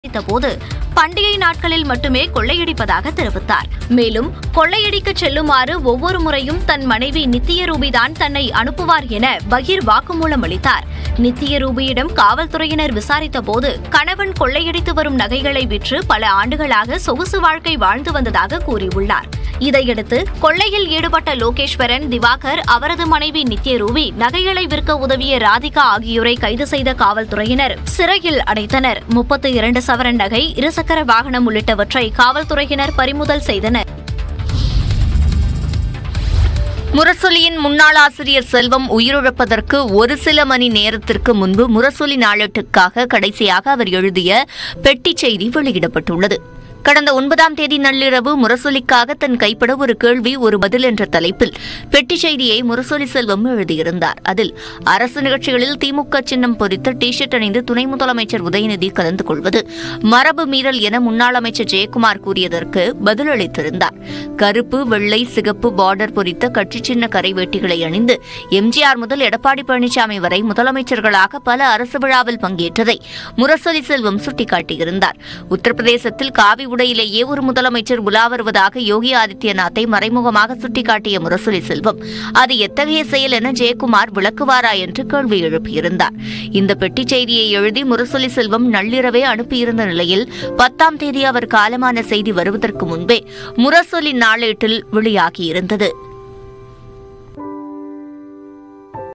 கணவனை கொள்ளையடிக்க அனுப்பிய மனைவி கைது | Theft | Thief
பண்டிகை நாட்களில் தமிழ்நாடு மட்டுமே கொள்ளையடிப்பது வழக்கம் - கொள்ளையன் திவாகர் - கொள்ளையடிக்கச் செல்லுமாறு ஒவ்வொரு முறையும் மனைவி தான் அனுப்புவார் - திவாகர் | Theft | Thief

11.10.24 SS

list=PLZjYaGp8v2I8Jl8PfHo2MQdWKs42-15cX